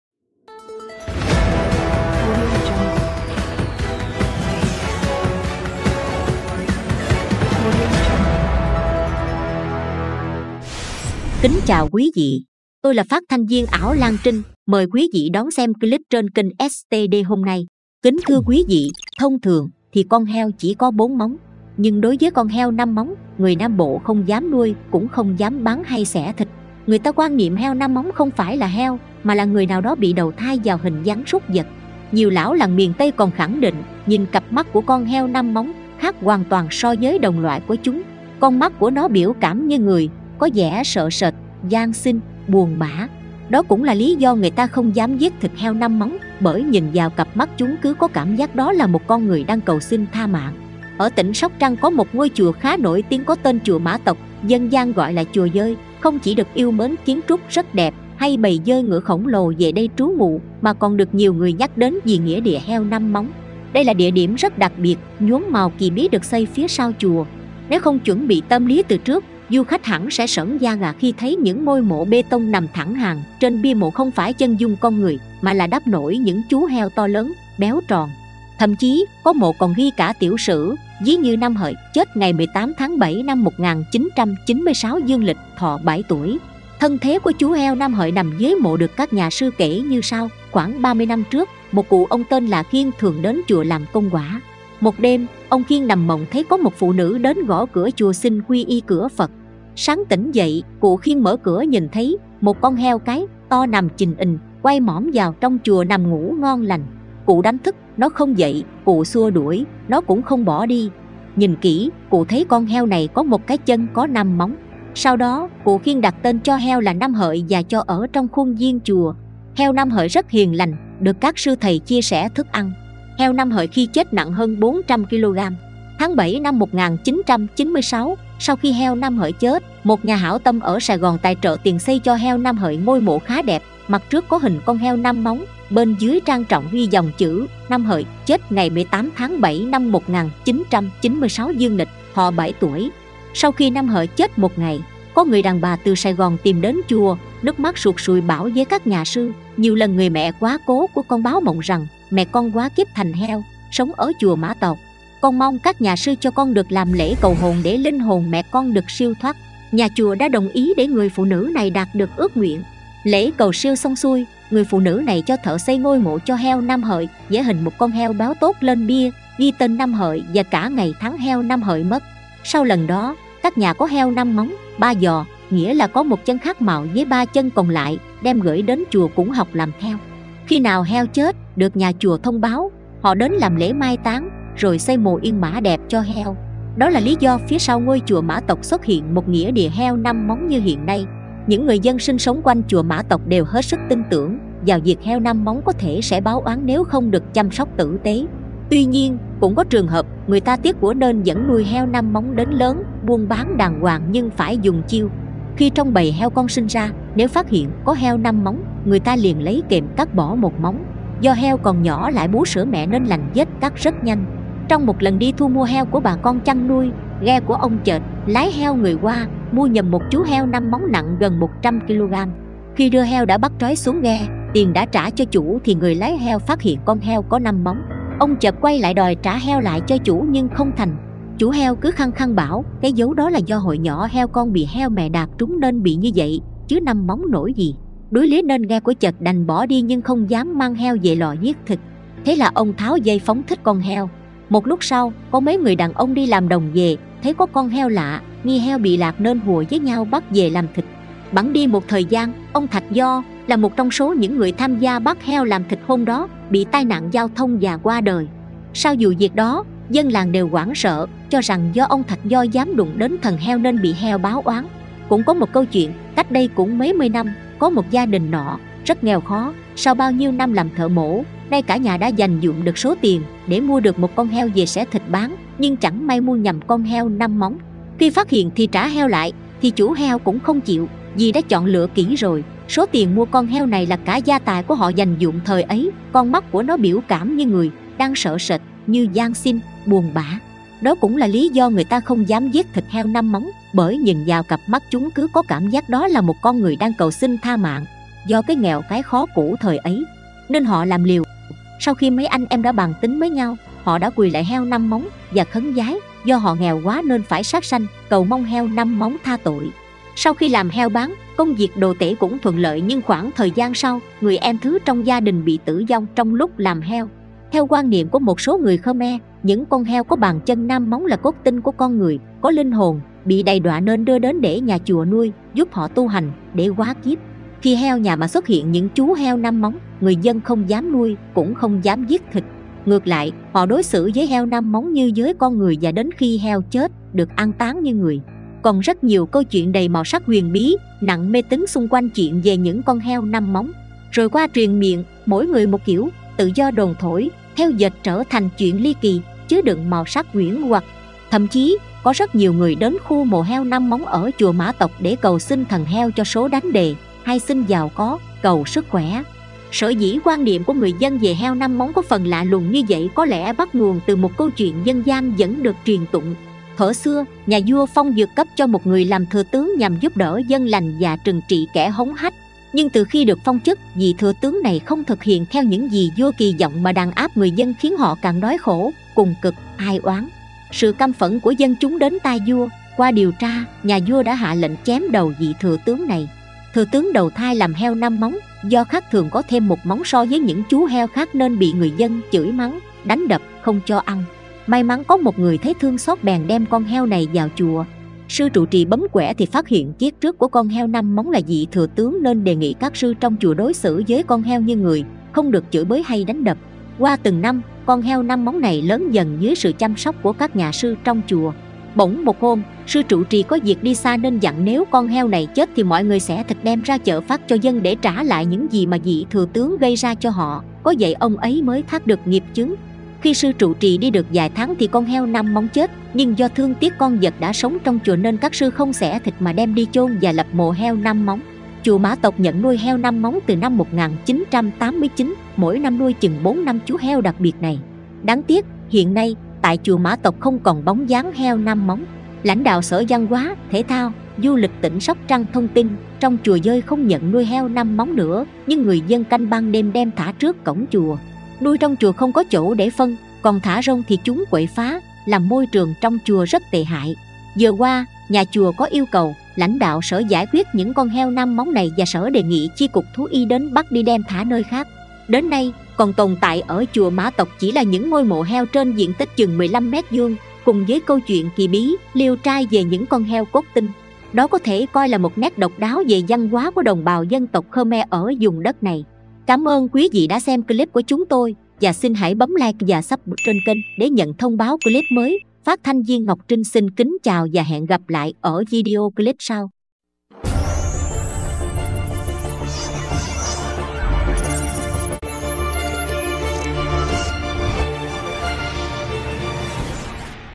Heo 5 móng và lời đồn Người chết vì bị “heo báo oán”
Thông thường, heo chỉ có 4 móng. Đối với heo 5 móng, người Nam Bộ không dám nuôi, cũng không dám bán hay xẻ thịt. Họ tin rằng heo 5 móng là “cốt tinh”, người hóa kiếp heo, nếu nuôi trong nhà sẽ quậy phá, sinh chuyện lục đục nên họ đem thả vào chùa để nghe kinh Phật tu hành và xóa được tội lỗi.
► Kenh STD
► Chuyên thực hiện các chương trình phim video theo yêu cầu: Live Stream Sự kiện, Ca nhạc, Khóa tu, Thuyết giảng...